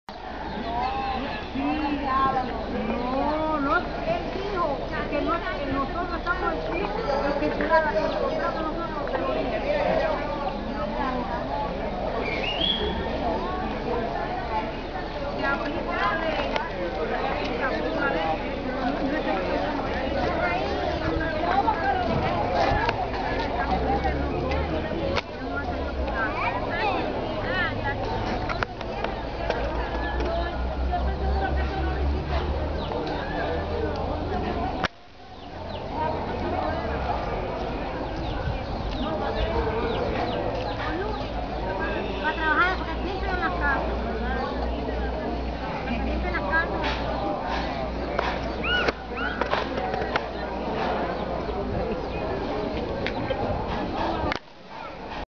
cuba-08-trinidad-valle de los ingenios
Daytrip to the Valle de Los Ingenios Trinidad with Cubatour